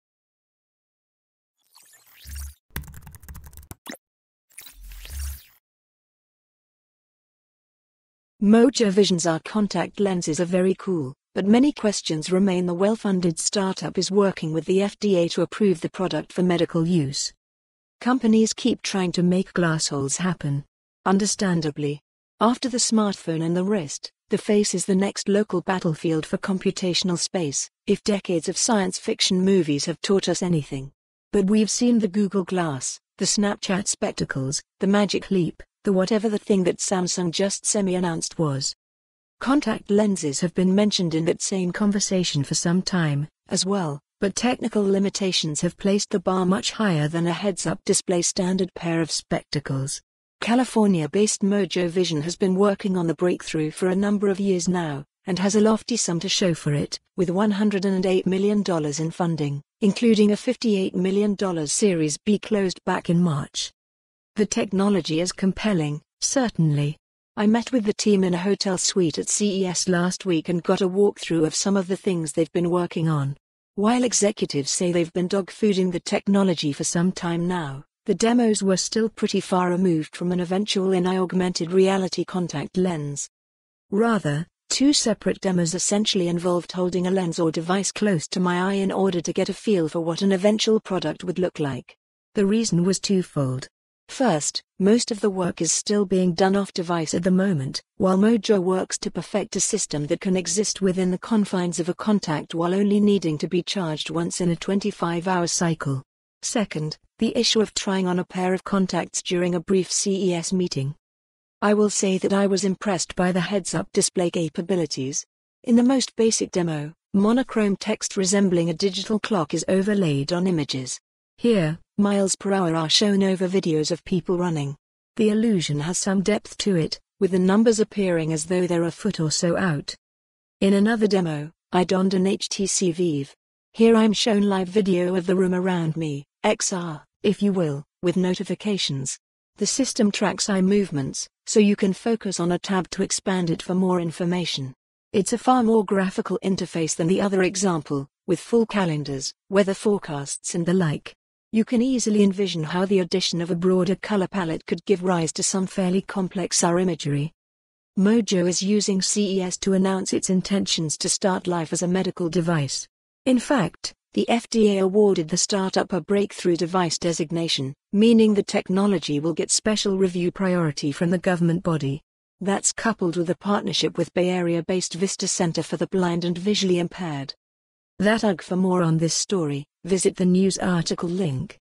Mojo Vision’s AR contact lenses are very cool, but many questions remain
The well-funded startup is working with the FDA to approve the product for medical use

Companies keep trying to make glassholes happen. Understandably. After the smartphone and the wrist, the face is the next local battlefield for computational space, if decades of science fiction movies have taught us anything. But we've seen the Google Glass, the Snapchat Spectacles, The Magic Leap, the whatever that thing that Samsung just semi-announced was.

Contact lenses have been mentioned in that same conversation for some time, as well, but technical limitations have placed the bar much higher than a heads-up display standard pair of spectacles. California-based Mojo Vision has been working on the breakthrough for a number of years now, and has a lofty sum to show for it, with $108 million in funding, including a $58 million Series B closed back in March.

The technology is compelling, certainly. I met with the team in a hotel suite at CES last week and got a walkthrough of some of the things they've been working on. While executives say they've been dogfooding the technology for some time now, the demos were still pretty far removed from an eventual in-eye augmented reality contact lens.

Rather, two separate demos essentially involved holding a lens or device close to my eye in order to get a feel for what an eventual product would look like. The reason was two-fold. First, most of the work is still being done off-device at the moment, while Mojo works to perfect a system that can exist within the confines of a contact while only needing to be charged once in a 25-hour cycle. Second, the issue of trying on a pair of contacts during a brief CES meeting.

I will say that I